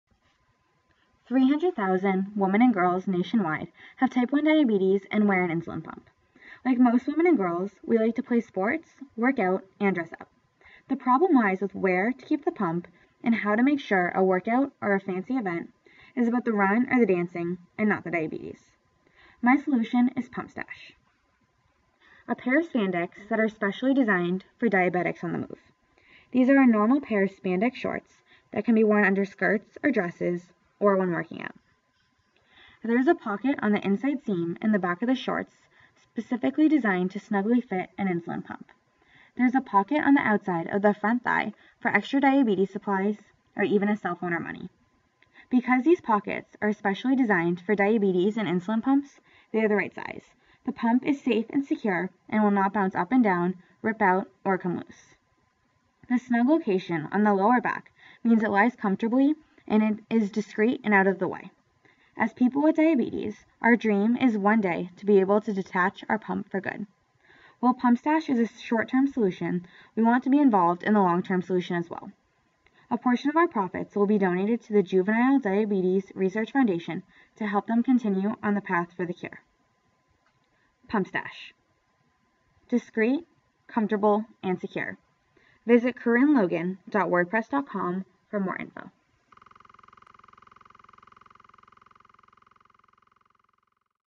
Corinne Logan, Pumpstash- Kenneth Cole Grant Finalist
Pumpstash compression shorts are designed to be a comfortable and secure way to hold insulin pumps for diabetics on the move